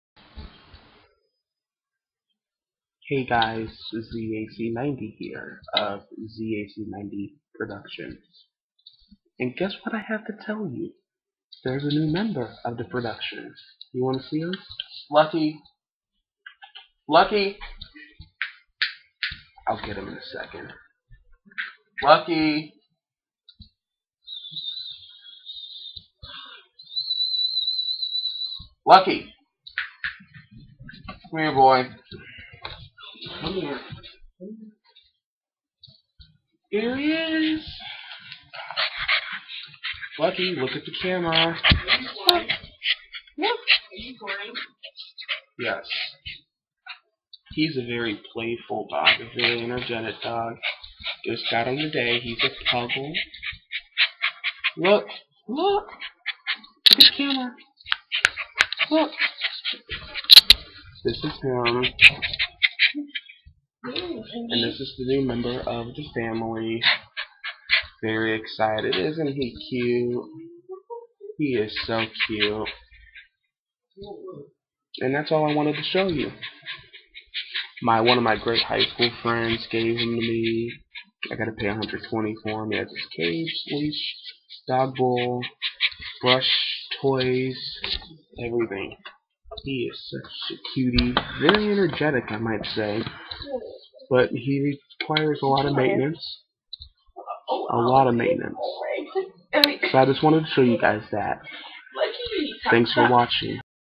My New Puppy Puggle "Lucky"
A short video of my very energetic puppy Lucky. He is only five months old, very well trained and tempermented.